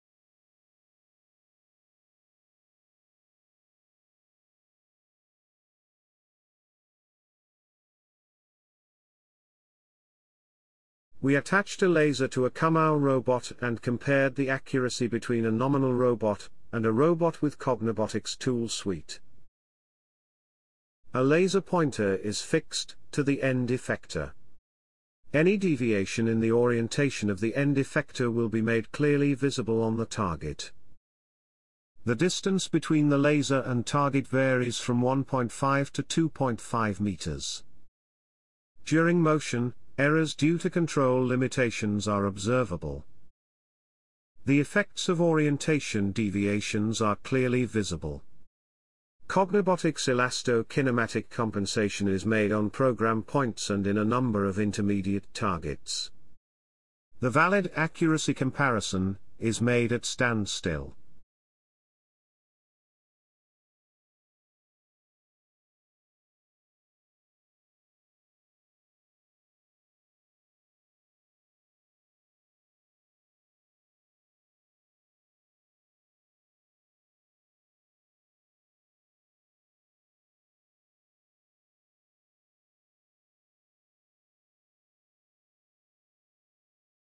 Cognibotics improves your robot accuracy with 700% with the Motion Precision Tool Suite
Our latest lab demo to visualize improvement examples with Cognibotics Motion Precision Tool Suite. Here showing 7x improved accuracy by compensating robot movements targets.